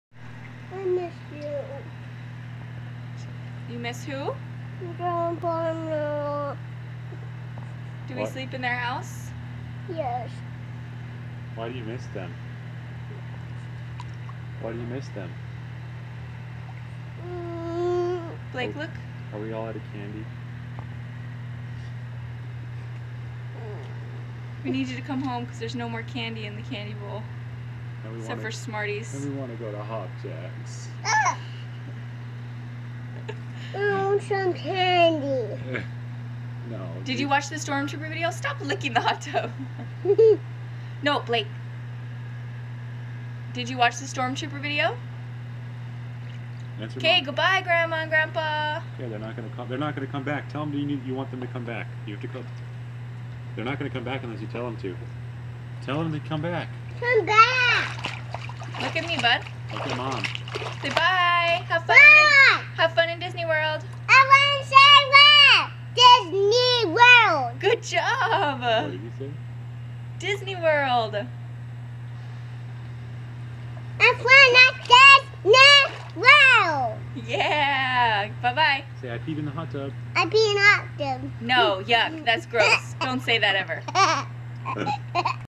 January 31, 2016
In the hot tub at Grandma and Grandpa Satko's house. We miss them!